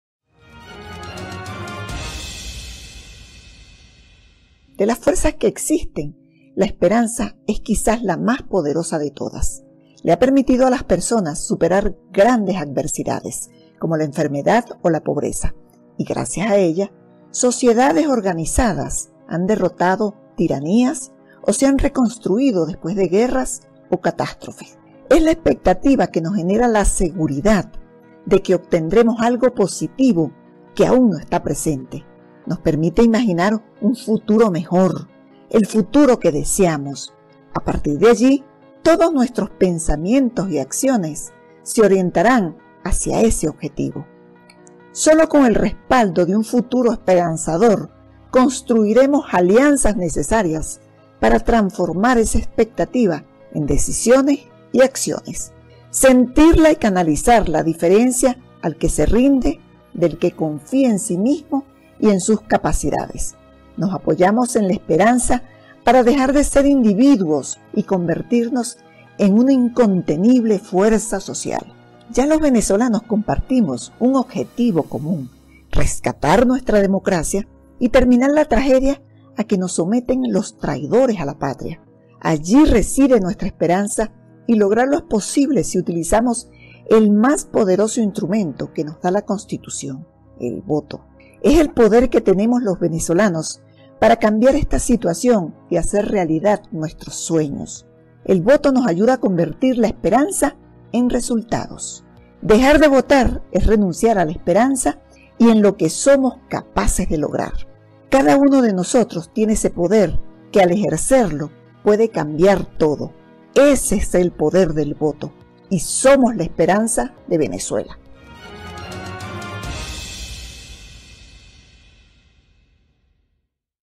SOMOS LA ESPERANZA DE VENEZUELA 5FEB Luisa Ortega Díaz
La esperanza es la fuerza más poderosa que nos motiva  a superar grandes adversidades. Nos permite proyectar un futuro mejor y generar los cambios que necesita el país. La esperanza de Venezuela está en sus ciudadanos!